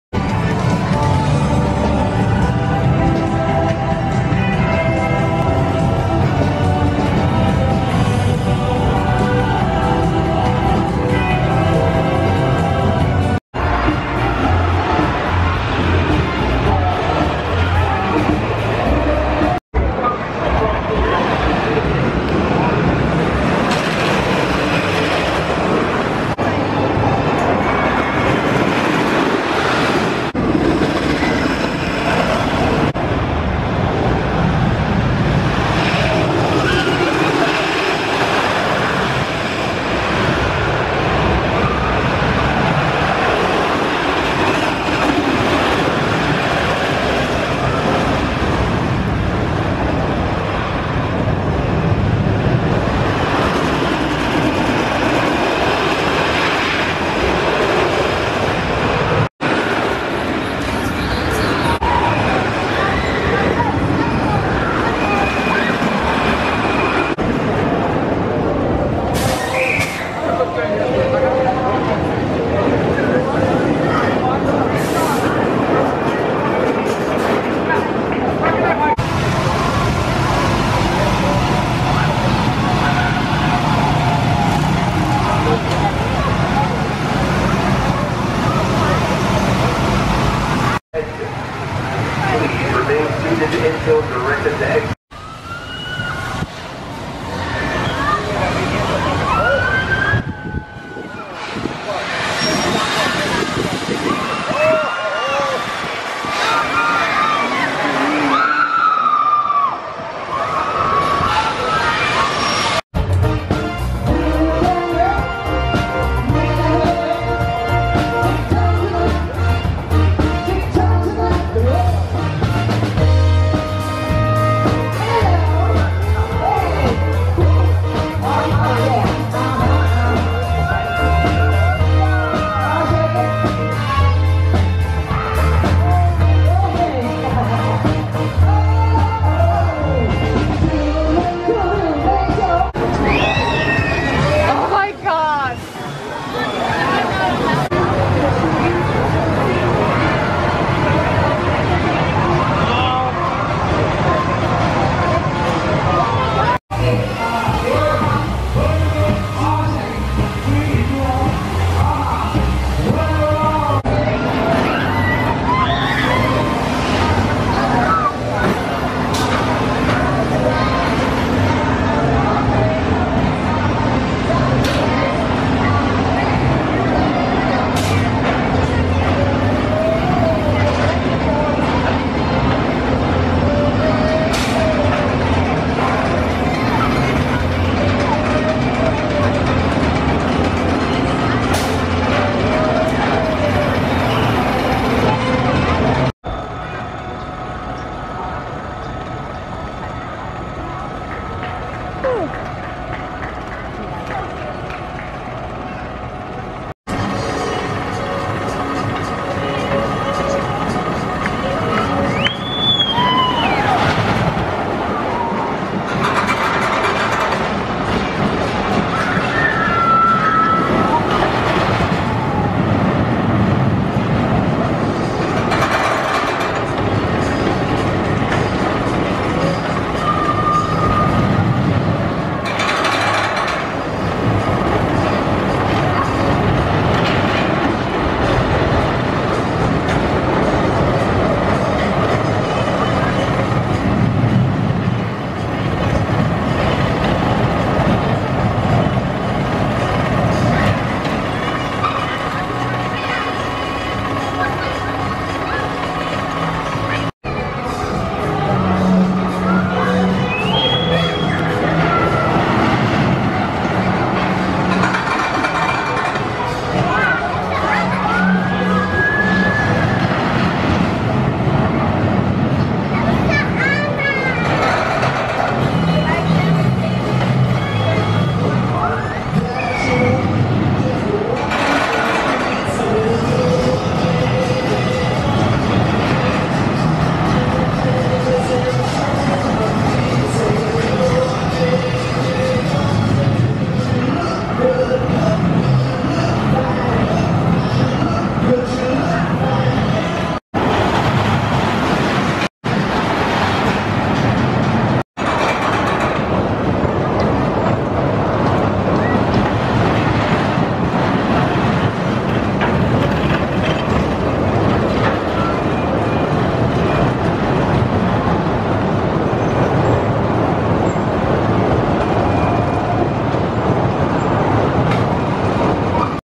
Fun Spot America Kissimmee Off-Ride Footage | Non-Copyright
Fun Spot Kissimmee is the best park in the Fun Spot chain.  This amusement park is home to Mine Blower, Hurricane, the world's tallest Sky Coaster, and some insane go kart tracks.

0:00- Park Overview
0:13- Mine Blower
1:07- Chaos Go Karts
1:36- Hot Seat
1:47- World's Tallest SkyCoaster
2:37- Screaming Eagles
2:44- Galaxy Spin (Rockstar Coaster)
3:17- Crazy Couch, Paratrooper, & Tilt-a-Whirl
3:27- Hurricane